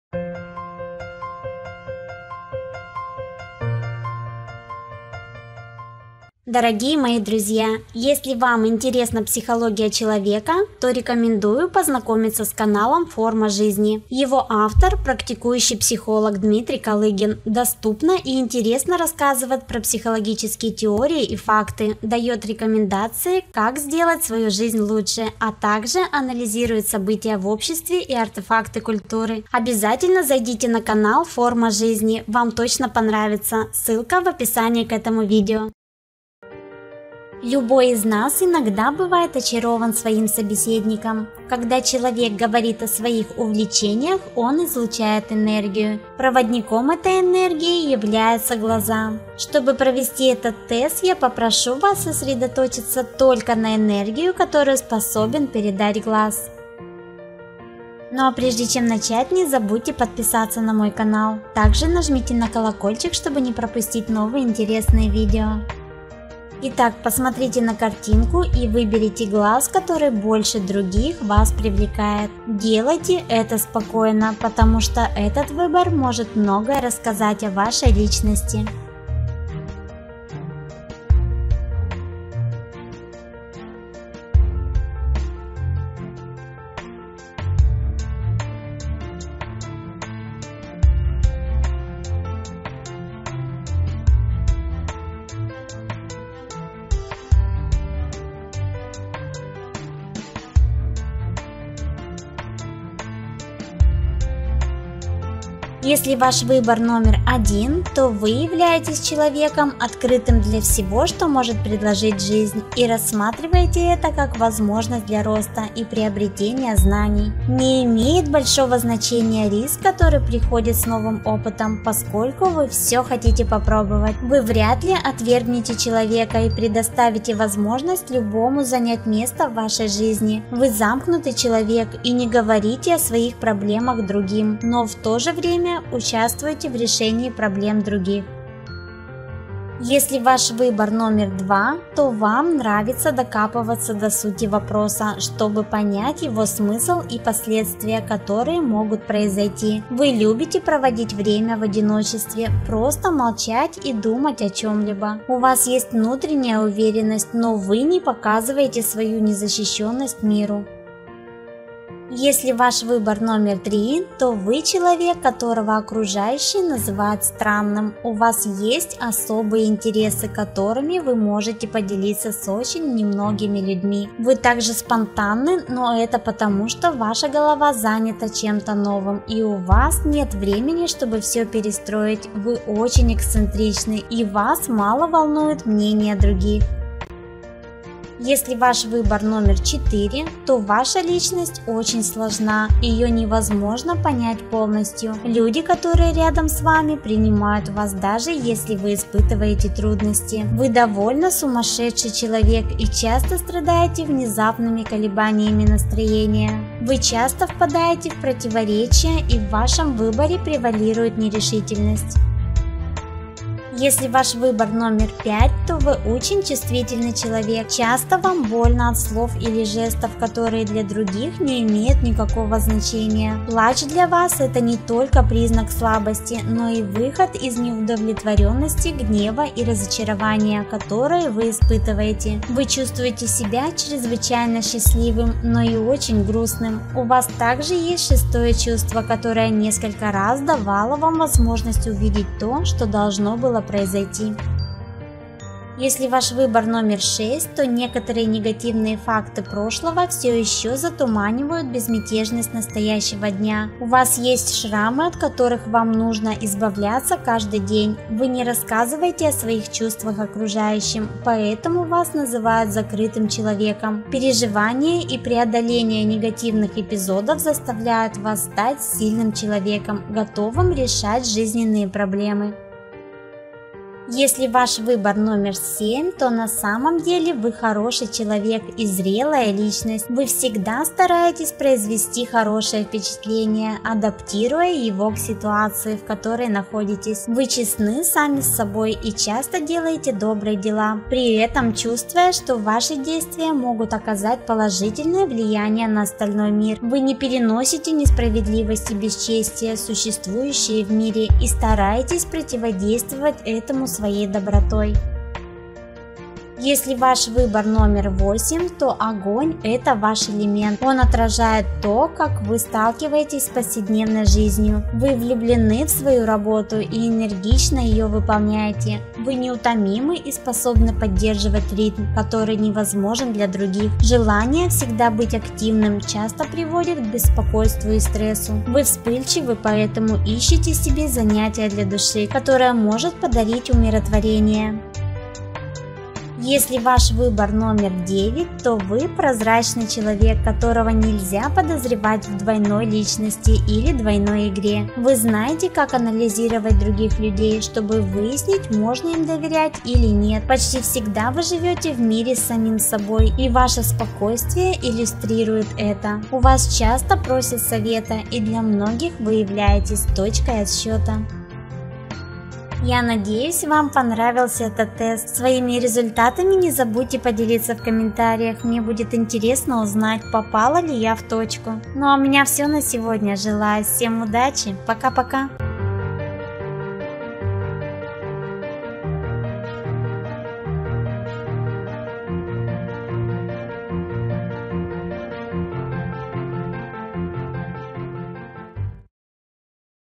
Тест ДЕВЯТИ ГЛАЗ расскажет о ВАС больше , чем ВЫ САМИ знаете о себе! Психологический тест онлайн!
Тест ДЕВЯТИ ГЛАЗ расскажет о ВАС больше , чем ВЫ САМИ знаете о себе! Психологический тест онлайн! Этот интересный Тест психологический Девяти Глаз: Выберите Картинку, Которая Вас Больше Всего Привлекает, И Узнайте Кое-Что О Своей Личности и о своем характере! Тест по картинке расскажет о вашем характере, о отношении к жизни и просто, я надеюсь, подарит хорошее настроение! Психологические тесты - это очень интересно, особенно интересно если это тесты онлайн или тесты картинка!